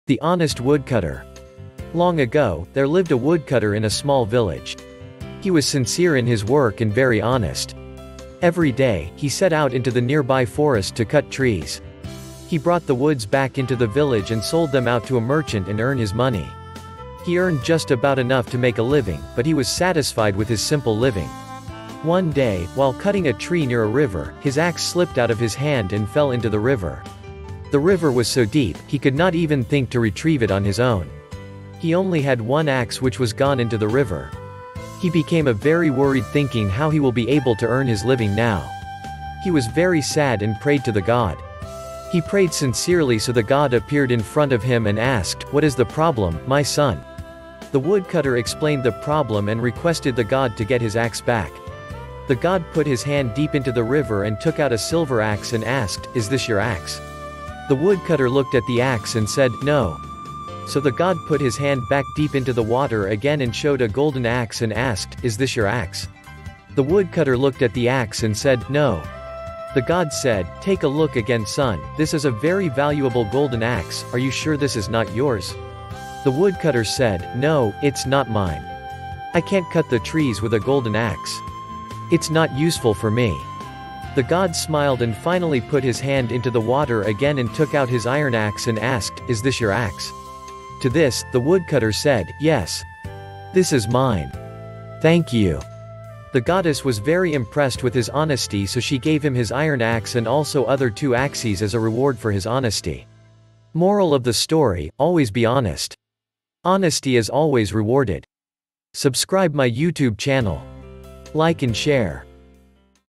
The Honest Woodcutter | Short Moral Story | Kids Story | Bedtime Story
The Honest Woodcutter : Long ago, there lived a woodcutter in a small village